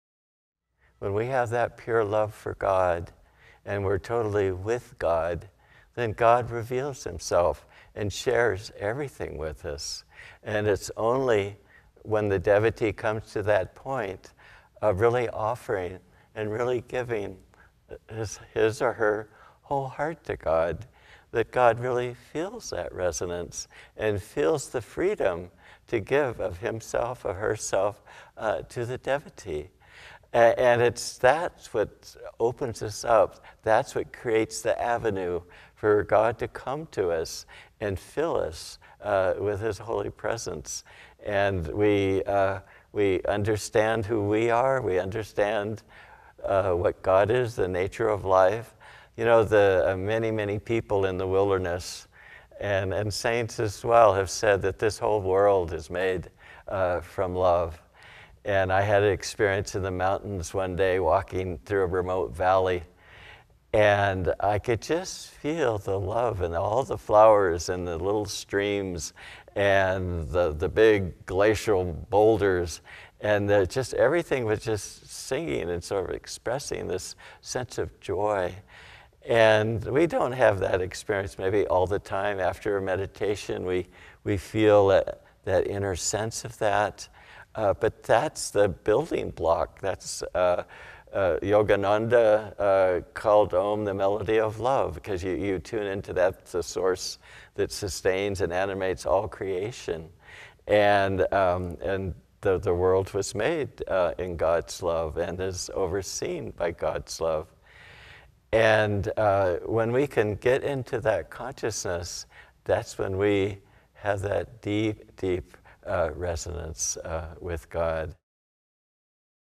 God is Love
Nayaswami Bharat shares this beautiful inspiration during his Sunday Service at Ananda Village, March 14th 2021.